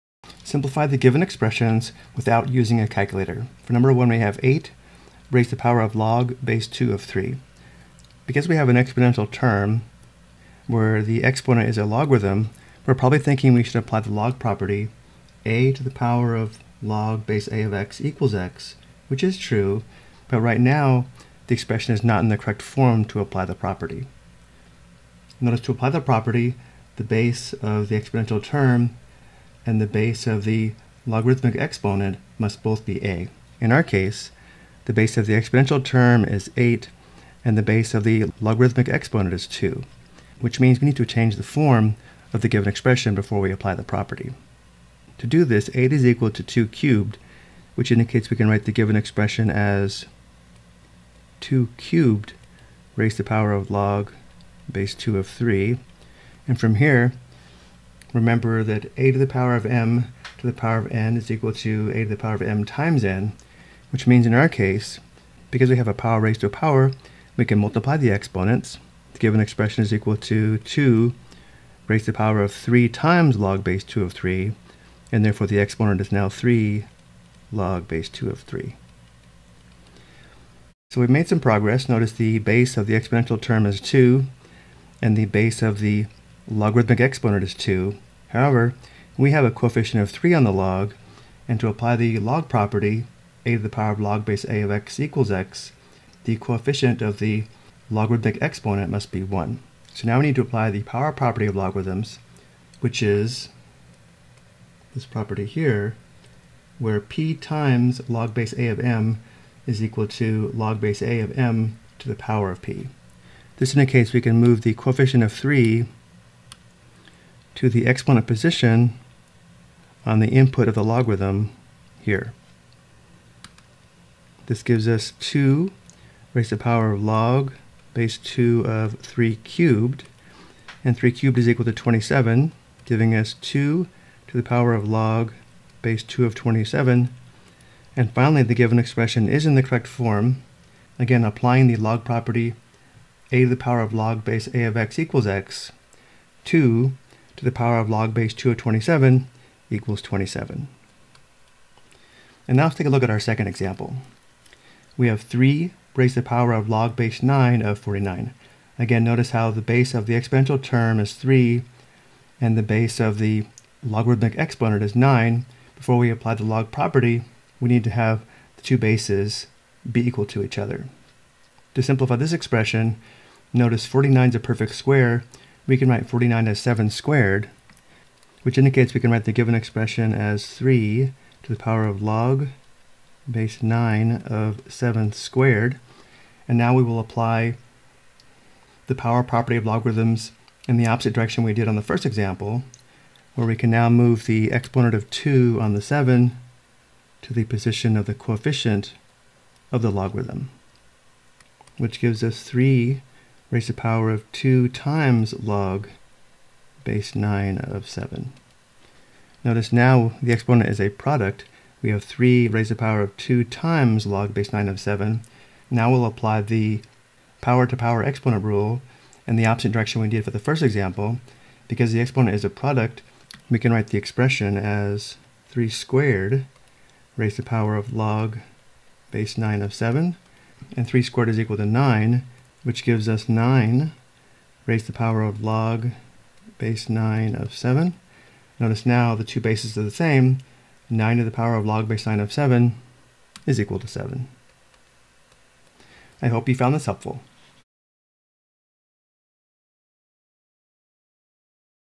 Simplify Expressions with Logarithmic Exponents Using Logarithmic Properties (Challenging)
This video explains how to simplify an expression with a logarithmic exponent using logarithmic properies.